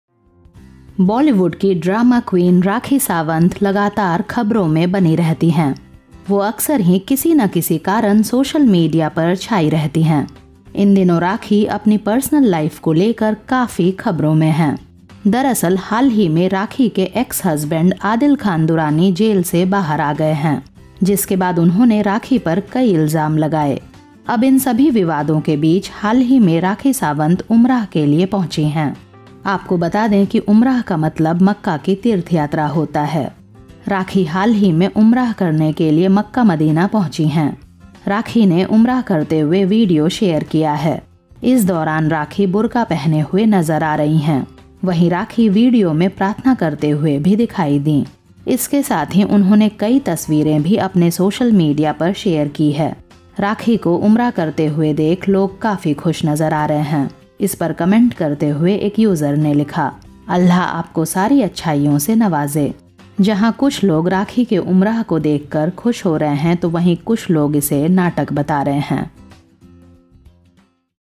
Rakhi Sawant Performs Her FIRST UMRAH At Makkah Madina
Popular personality Rakhi Sawant aka Fatima, who jetted off to Saudi Arabia to fulfill her dream, has finally performed her first Umrah. Several photos and videos of her from Masjid-al-Haram in the holy city of Makkah have emerged online and are going viral like wildfire. She is accompanied by her Rakhi brother, Waahiid Ali Khan, and his wife, Shaista Ali Khan.

Rakhi Sawant jetted off on Friday morning (August 25). She first visited the holy city of Madinah.